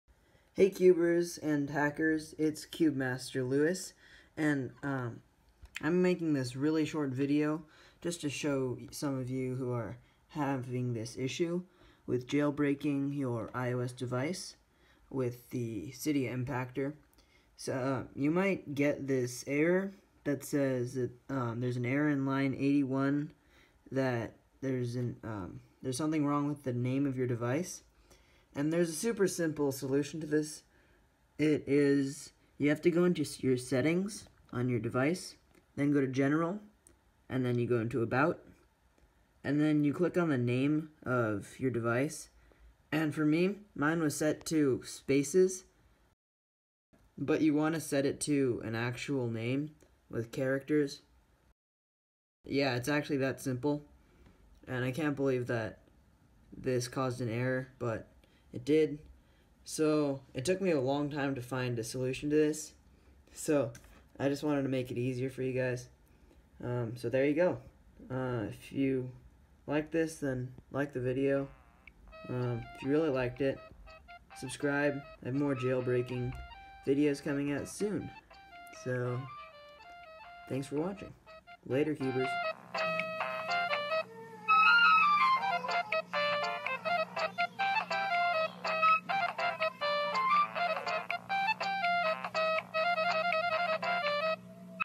Cydia Impactor Error Solution - file: provision.cpp; line 81; name
In this video I walkthrough a simple solution to an annoying Cydia Impactor bug that prevents the iPhone from being jailbroken because of a Name Error